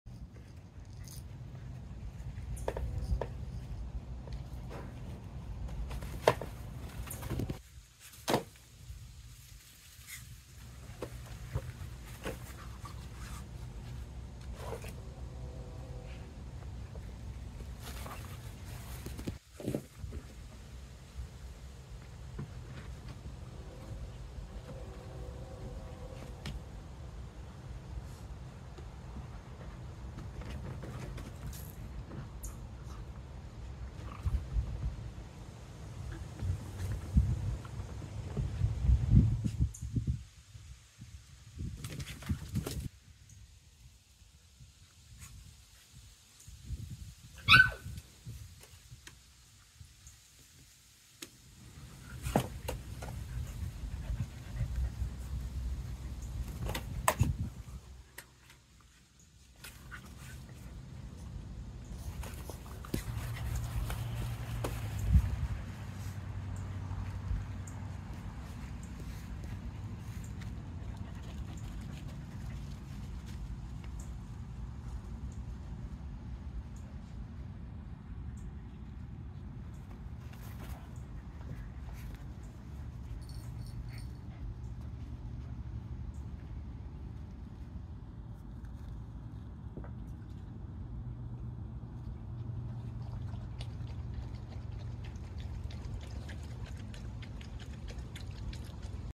Darby and Ollie playing tag
Darby X Vic litter